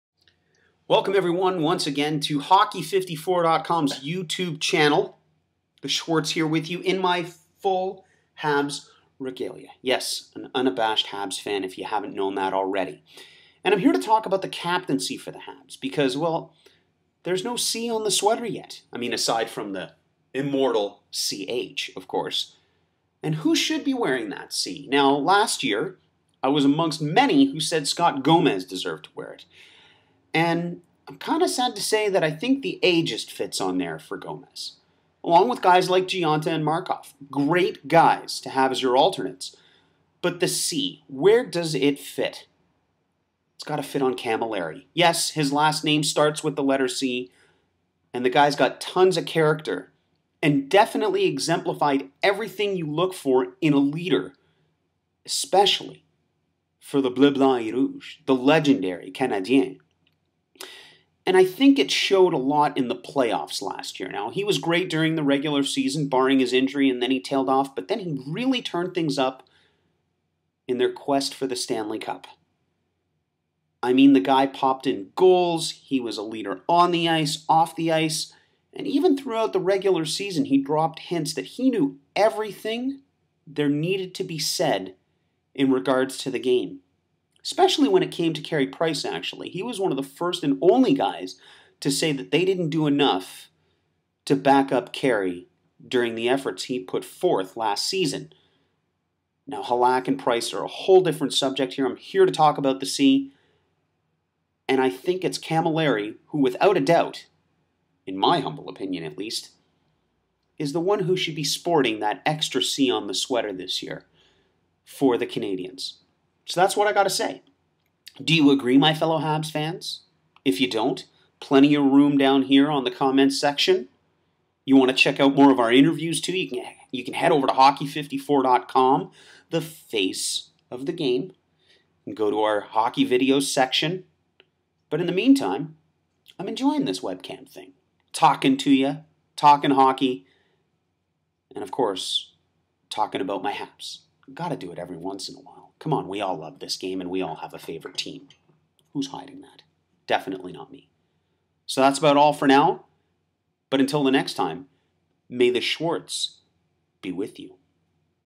Hockey54.com - Put The 'C' On Cammalleri
Hockey54.com's Launy "The" Schwartz is having fun with the webcam. His topic today? That Mike Cammalleri should have another 'C' on his sweater this season with the Montreal Canadiens. Hey, The 'C' already fits with Cammalleri's last name, one more won't hurt.